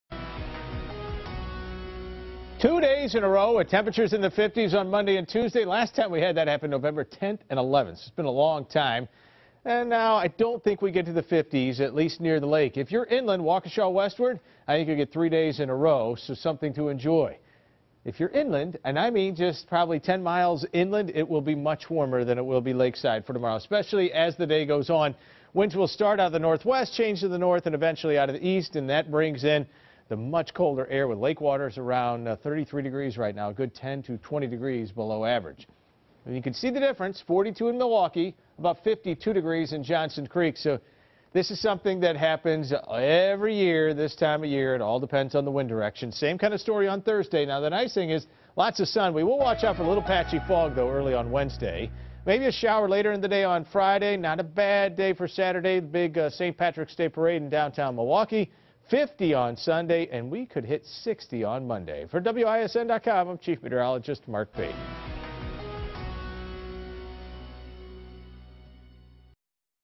Watch Mark Baden's Forecast
Here's the latest Weather Watch 12 Forecast with Chief Meteorologist Mark Baden.